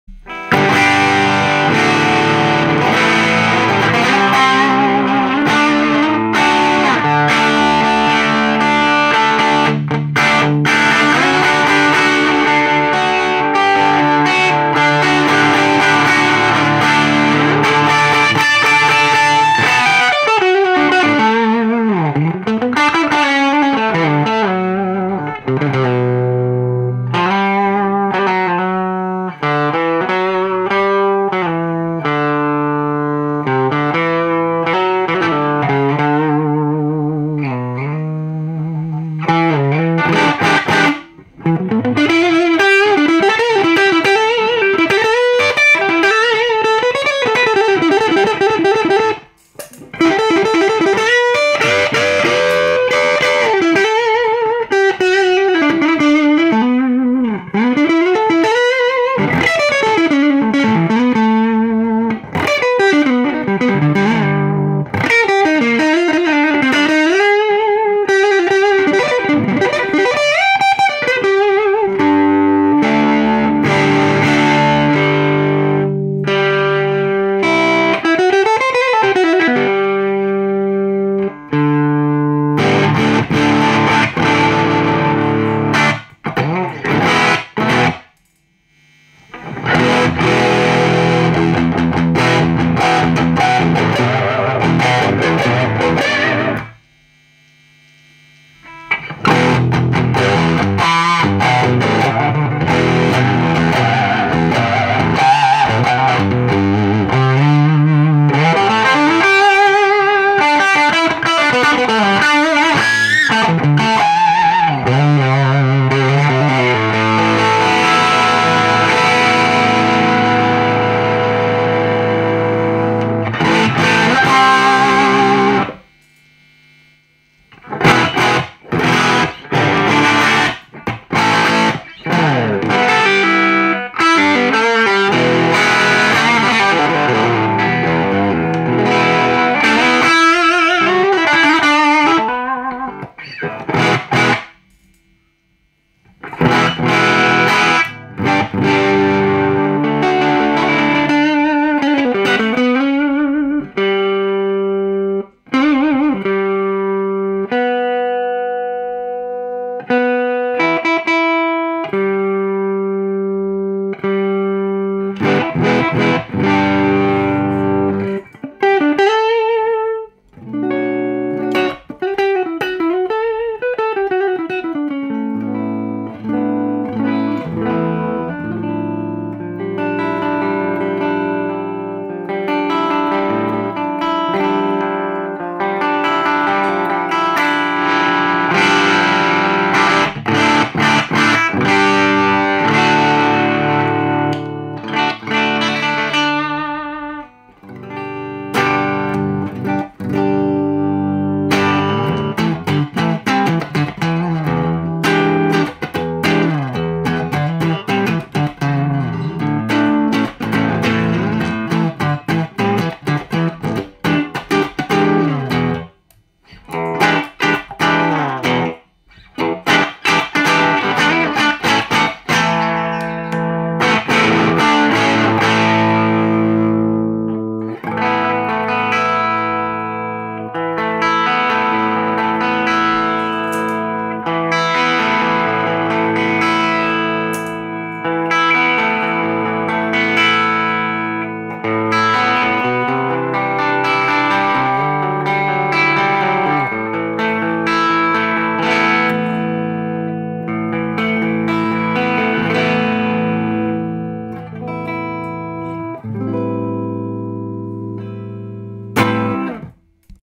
Ceriatone Overtone Special HRM Bluesmaster 50W Over Drive Channel ①
Ceriatone Overtone Special HRM Bluesmaster 50W ドライブチャンネルです。リアパネルにあるOD Trimはセンターにセット。ギターはオリジナルLPタイプ、シールドはBold Cable Fatを使用。